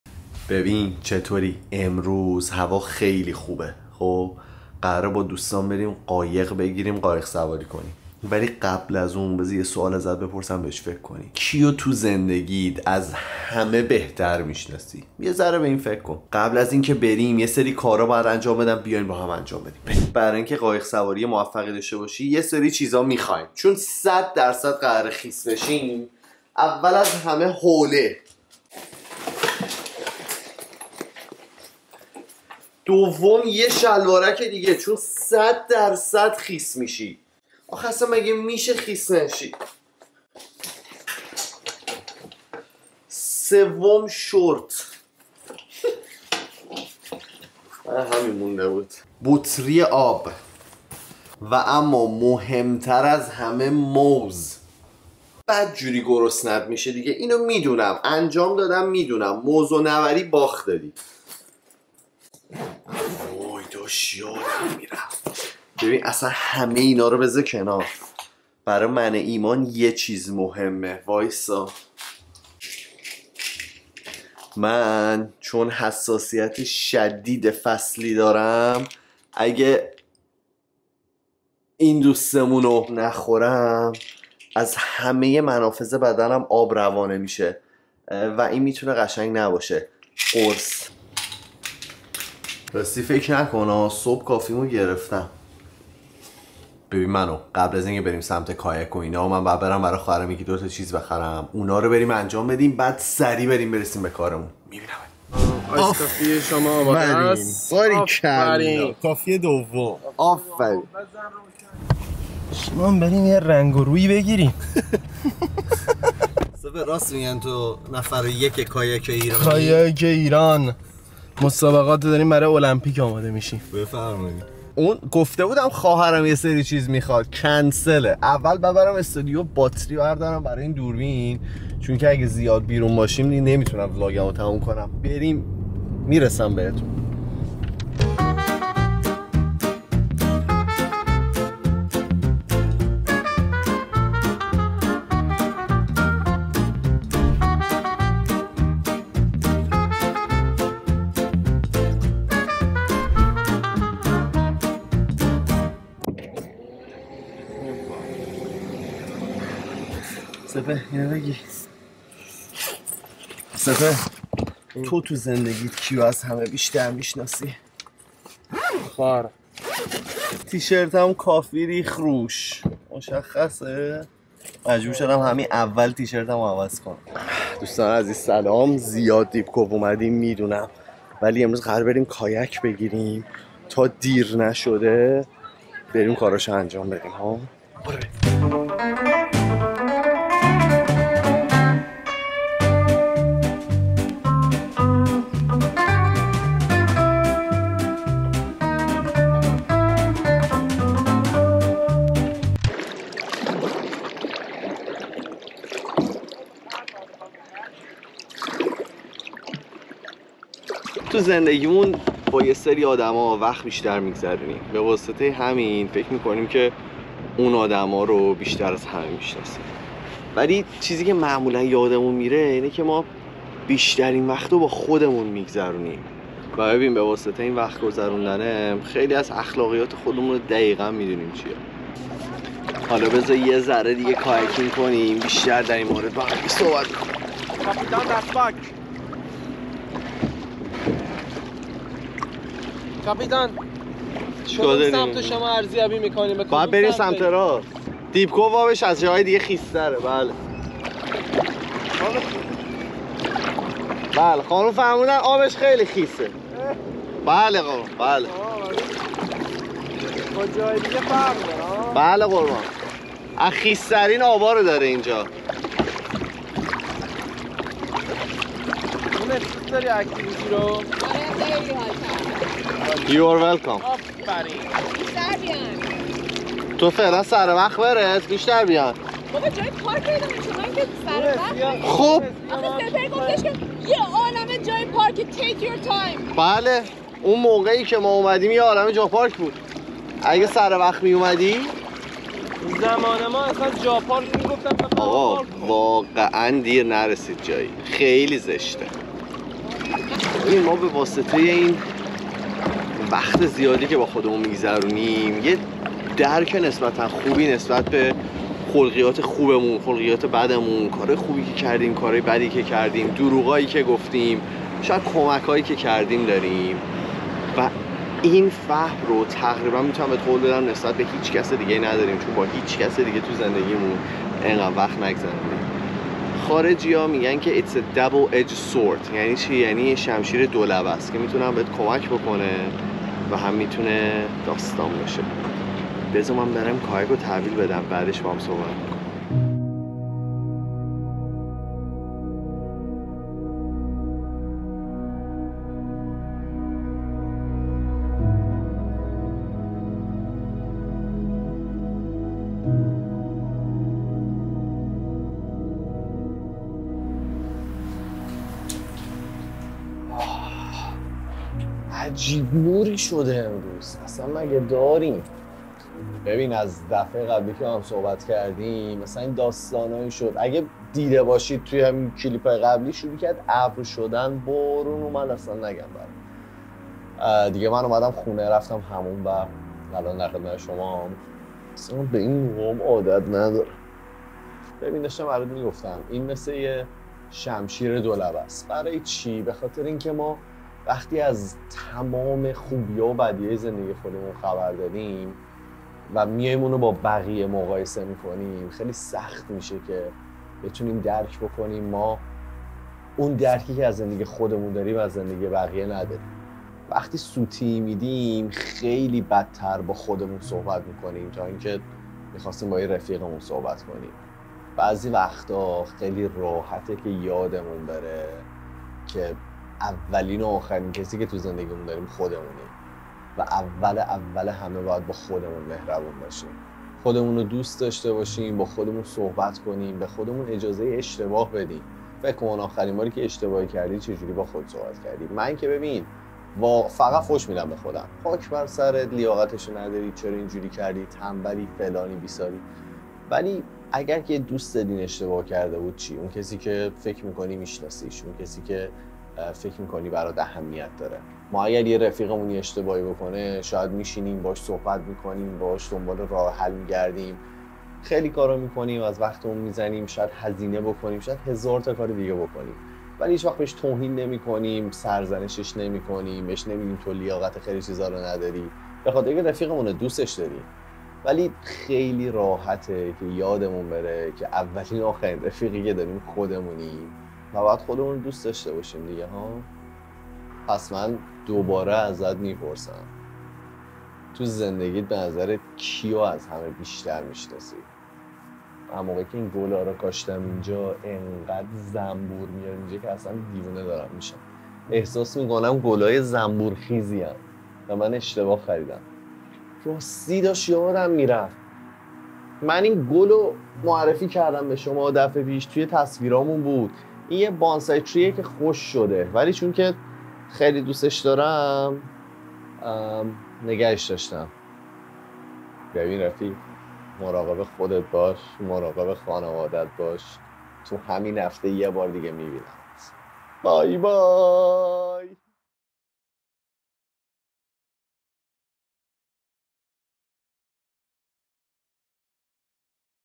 بهترین رفیقت کیه؟
امروز با چند تا از دوستان رفتیم قایق گرفتیم و رفتیم تو آب و یکم در مورد رفاقت و دوستامون حرف زدیم

🎬 Video Credits:
Editor: @MikaDart
Graphics: @MikaDart

🎥 Chapters:

00:00 برنامه روز
00:20 وسایل مورد نیاز
01:45 سپهر آمد
02:40 کیو از همه بیشتر میشناسی؟
03:05 Drone Shots - Deep Cove
03:25 کایاک بازی
04:00 کاپیتان دست پاک
05:00 با خودت بدی؟
06:25 شمشیر ۲ لبه
08:21 درختم